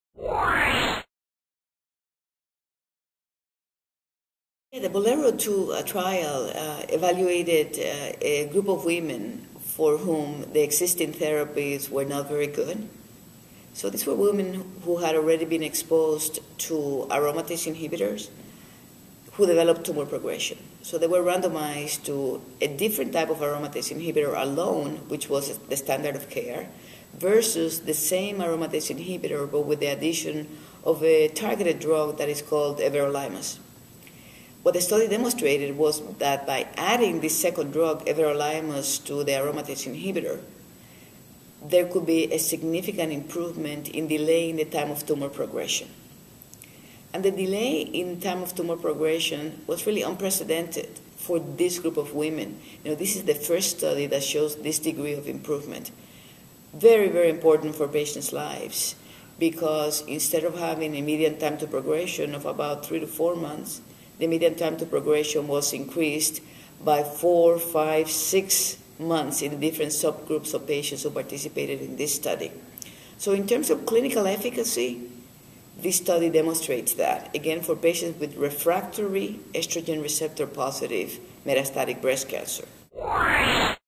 Dr. Perez Discusses the BOLERO-2 Everolimus Trial
Edith A. Perez, MD, deputy director, Mayo Clinic Cancer Center, Florida, director, Breast Program, Serene M. and Frances C. Durling Professor of Medicine, Mayo Medical School, discusses the results of the phase III BOLERO-2 trial that examined women with hormone receptor-positive (HR+) metastatic breast cancer that had developed disease progression and resistance to aromatase inhibitors.

The trial enrolled 724 postmenopausal women that had progressed on anastrozole or letrozole. Patients were randomized 2:1 to receive exemestane plus everolimus or exemestane and placebo. The addition of everolimus resulted in a median progression-free survival of 7.4 months compared to 3.2 months in the control arm.

Perez describes the results of the BOLERO-2 trial as unprecedented. The addition of everolimus showed clinical efficacy and offers immense value for patients with refractory estrogen receptor positive metastatic breast cancer.